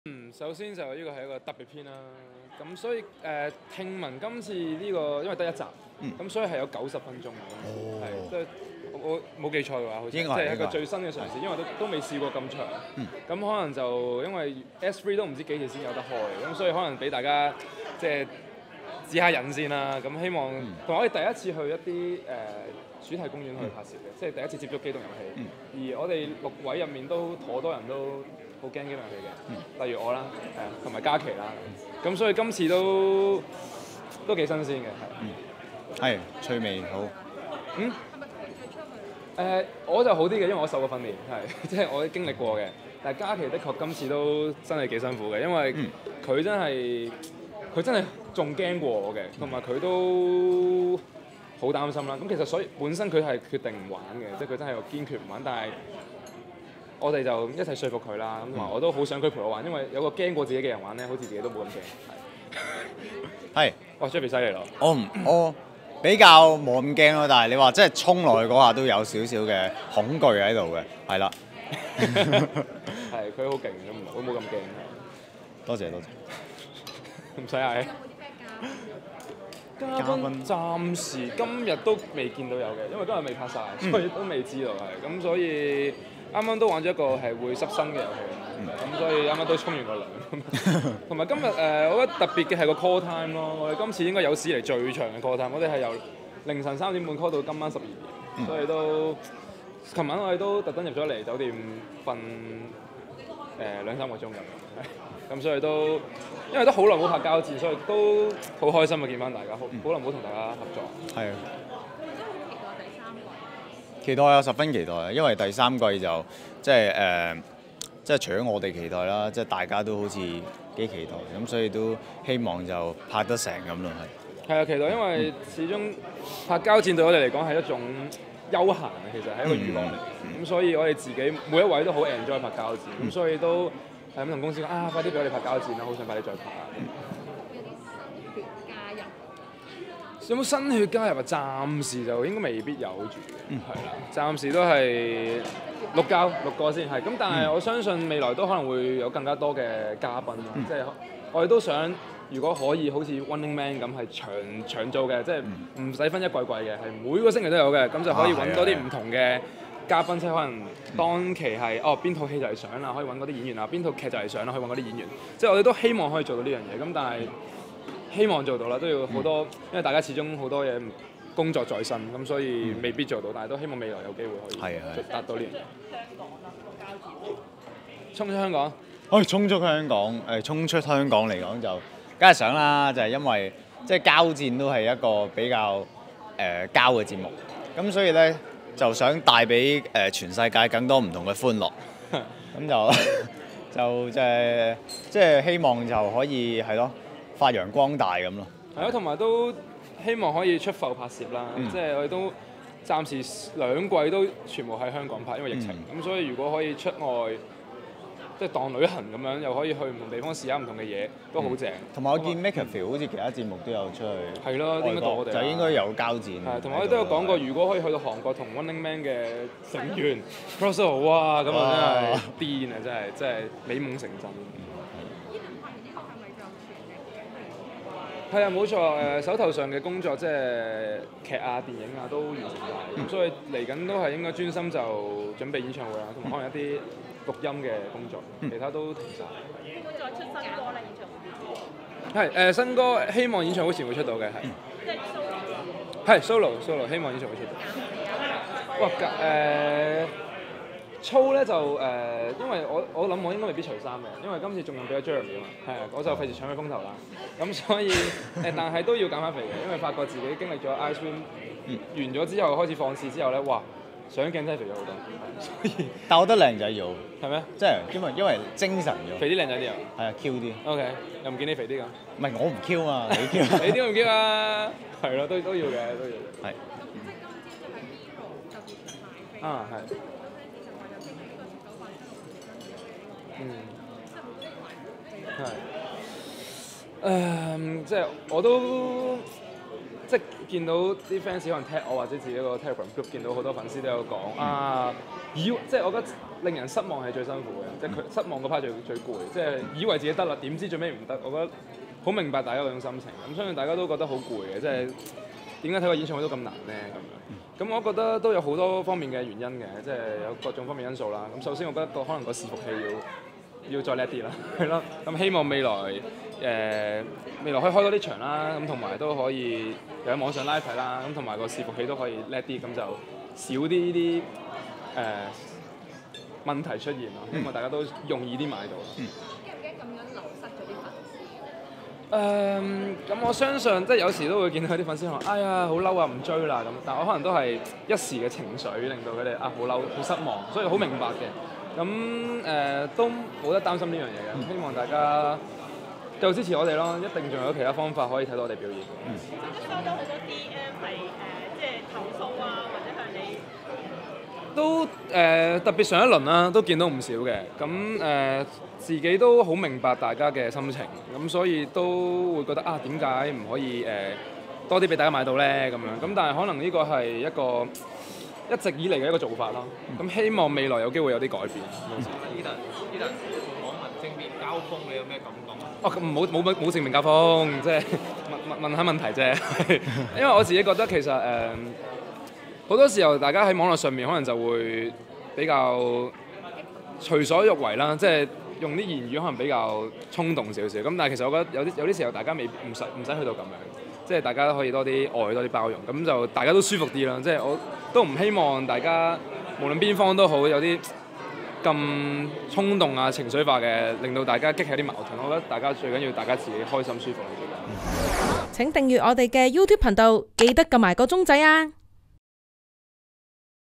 《膠戰SSP》｜MIRROR演唱會一票難求 Edan明白大家失望的心情：點解唔俾大家買多啲飛
《膠戰》6位膠人今日齊齊到海洋公園拍攝《膠戰 SSP》—《膠戰盛夏祭！爭奪高級浴衣大作戰！》，Edan(呂爵安)與Jeffrey(魏浚笙)兩兄弟齊聲表示渴望拍《膠戰 S3》，並且長拍長有，邀請不同嘉賓玩遊戲，他希望有天《膠戰》能衝出香港，到韓國與《Running Man》大玩crossover，但暫時以90分鐘的特別版讓大家止止渴。Edan表示6位膠人首次往主題公園拍攝，大爆珈其比他更害怕玩機動遊戲，Jeffrey就保持無所畏懼之態，呼籲膠粉密切留意。

Edan透露，完成《膠戰 SSP》拍攝後，將會全面投入錄新歌和籌備演唱會，他笑言今次不會騷肌，以免搶了Jeremy(李駿傑)的風頭，而且拍完《i.SWIM》後已甚少操練，比最fit時期相距胖了不少，不過Jeffrey就讚Edan胖一點更可愛重難木日

談到MIRROR演唱會一票難求，Edan認真表示，明白大家心情，「失望係最辛苦，以為自己買到，點知最後買唔到，明白大家心情，覺得好攰，點解睇個演唱會都咁難。」問到可擔心粉絲流失？他直言，「聽到有粉絲話好嬲，唔追喇，明白他們失望，但也冇得擔心，希望大家繼續支持。」他表示也收到DM投訴，深感無奈，「點解唔能夠俾大家買多啲飛，可能這是一直以來的做法，希望未來有改變。」最後，談到回應網民惡言時，他認為不少人在網絡上隨所欲為，言語衝動一點，希望可以少點矛盾。

bmgproductionmusic